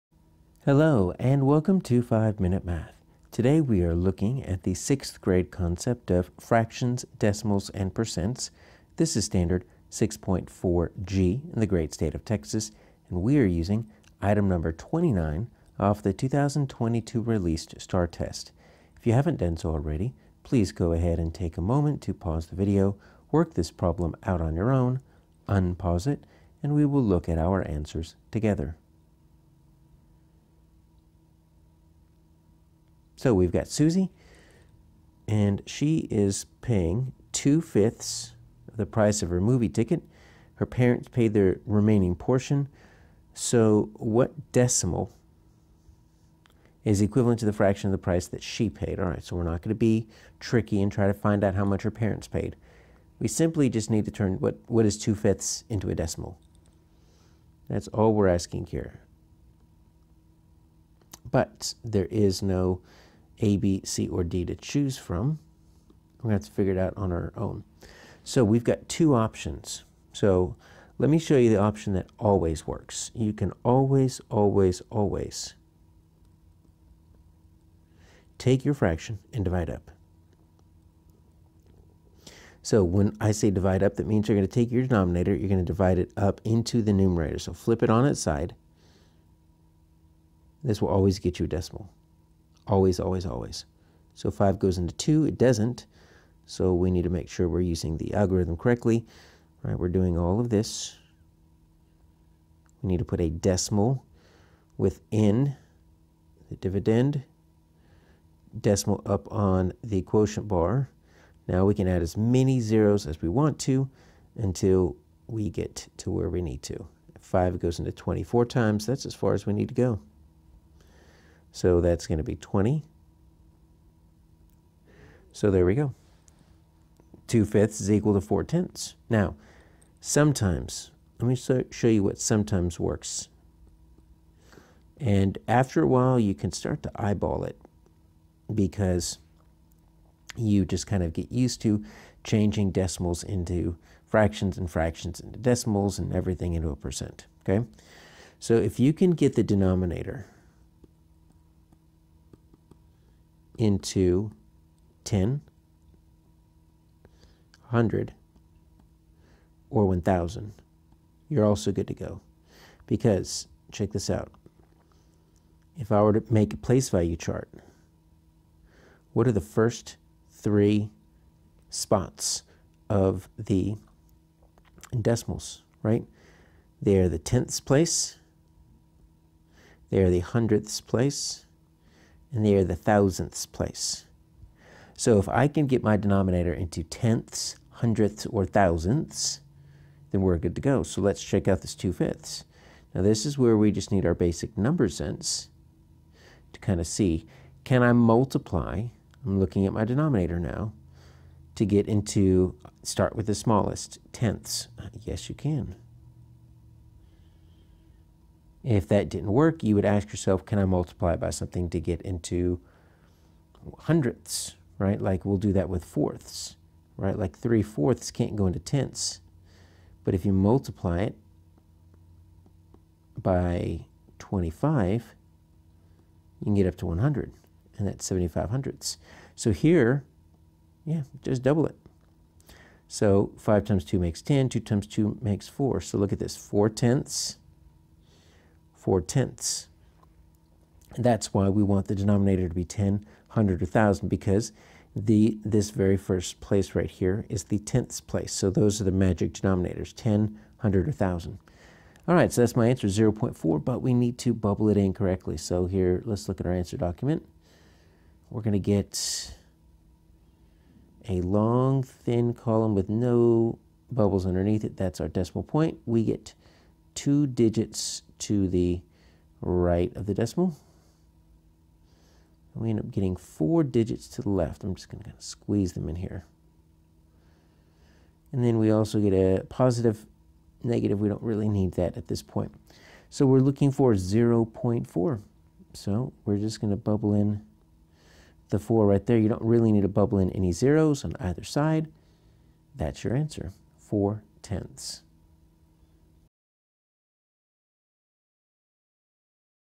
6th Grade STAAR Practice Equivalent Fractions, Decimals, and Percents (6.4G - #10)
Practice generating equivalent forms of fractions, decimals, and percents using real-world problems, including problems that involve money, with a test item from the 2022 released STAAR test (#29 - 6.4G).